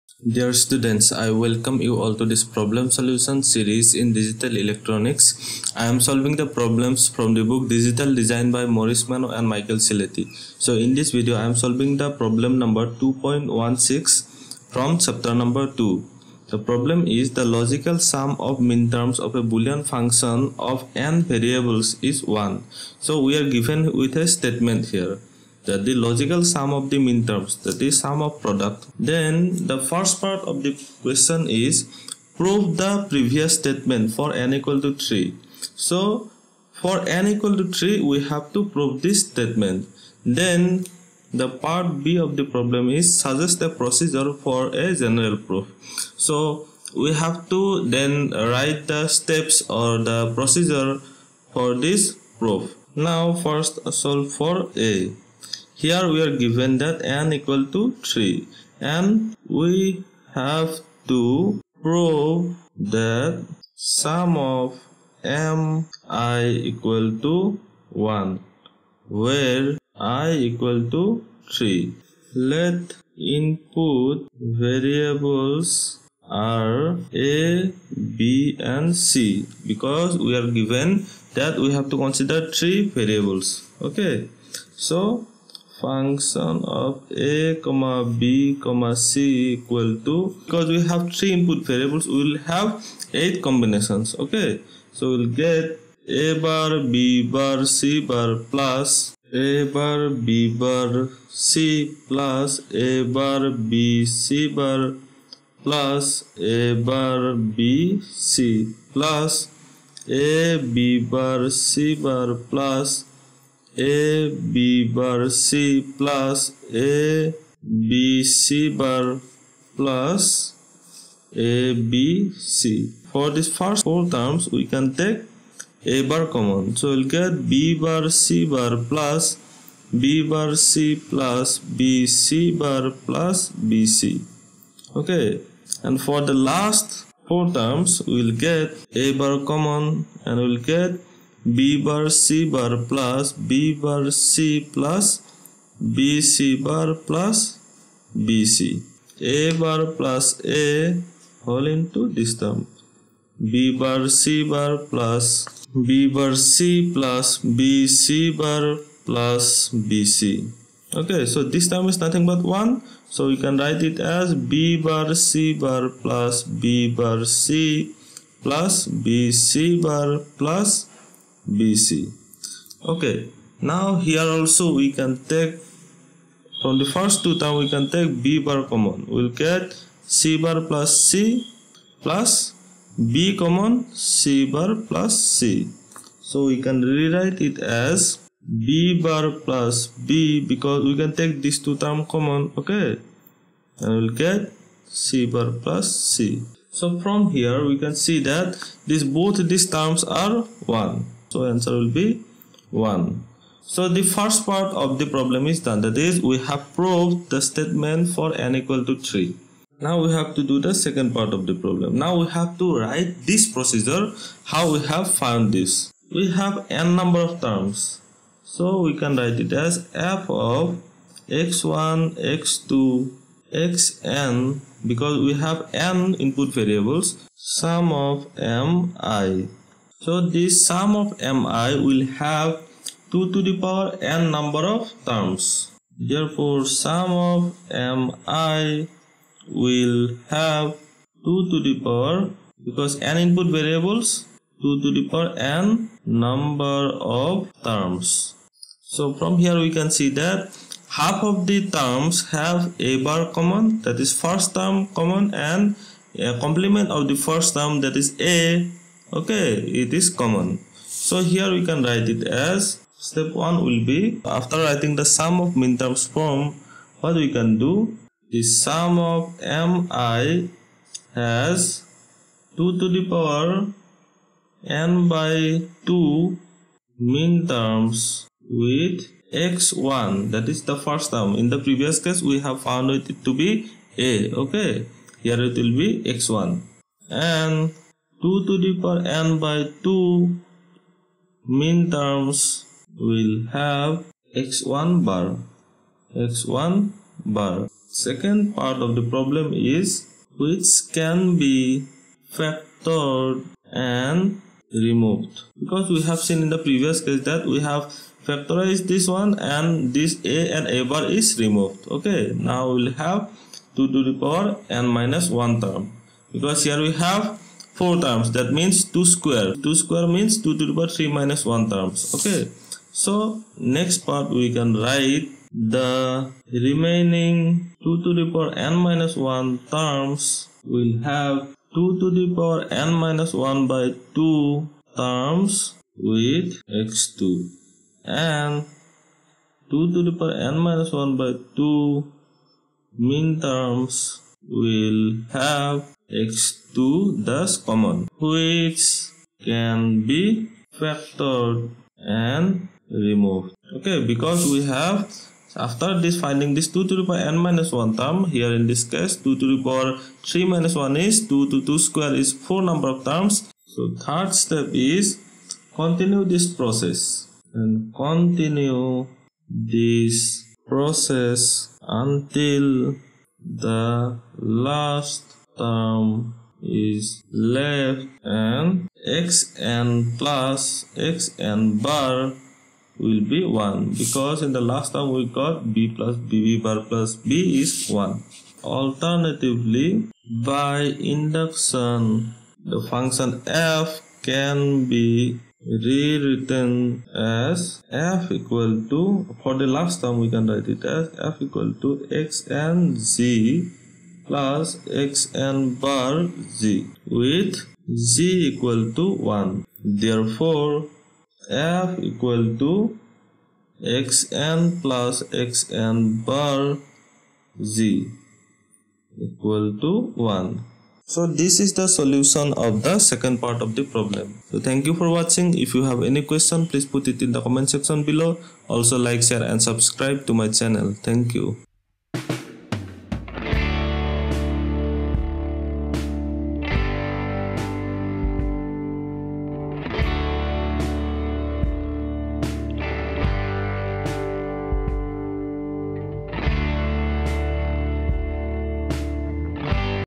Q. 2.16: The logical sum of all minterms of a Boolean function of n variables is 1. (a) Prove the
Q. 2.16: The logical sum of all minterms of a Boolean function of n variables is 1. (a) Prove the previous statement for n = 3.
(b) Suggest a procedure for a general proof.